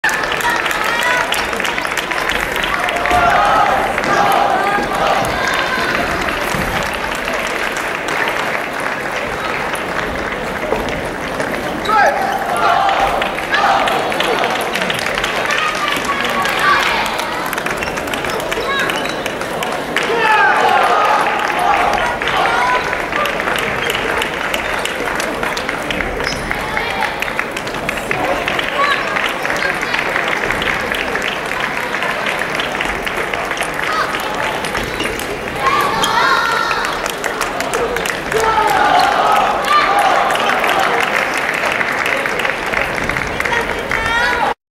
Lovely table tennis players in Japan
You can't miss their games for everything. The games was being played between beauty and beauty in Japan! Ms.Yamaguchi wearing yellow uniform is a good Japanese attacker likewise DANG Ye Seo(Korea).